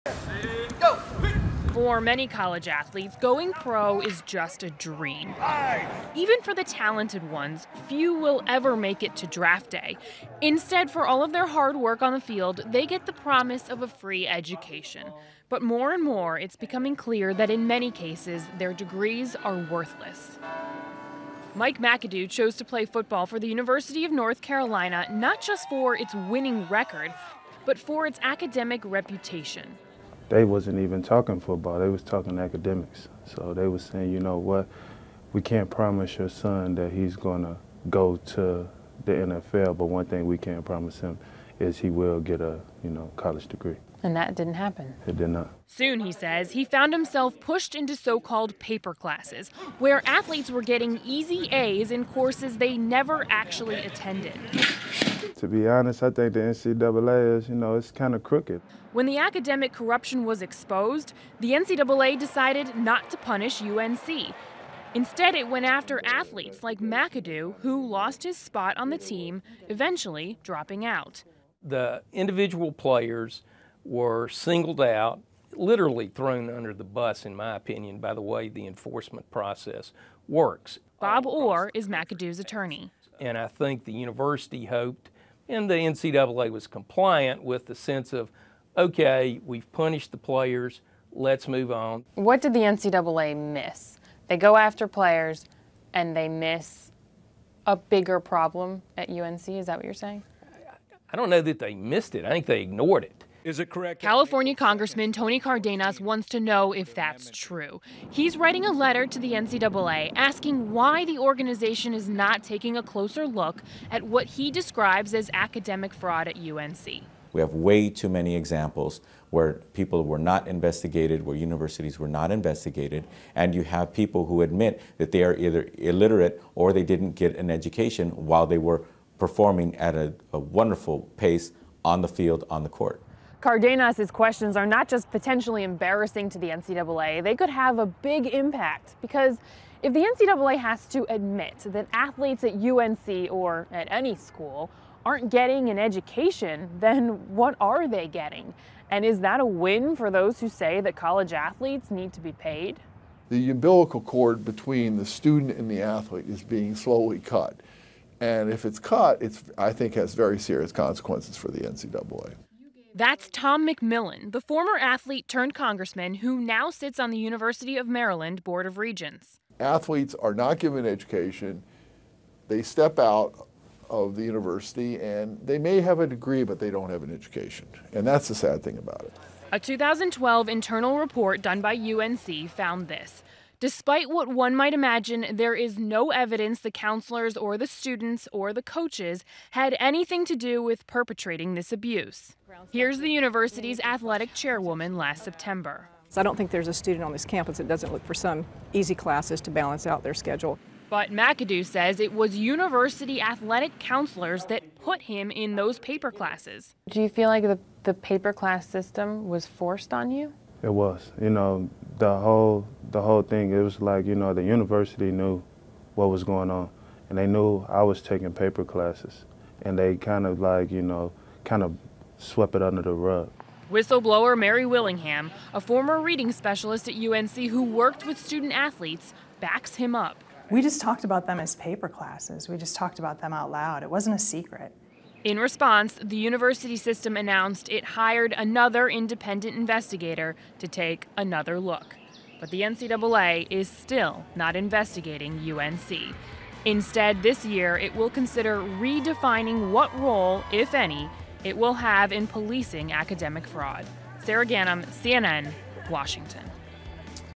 Under-education a painful reality for undrafted athletes
With recent reports uncovering poor education for student-athletes, Sara Ganim looks at life for those who don't go pro.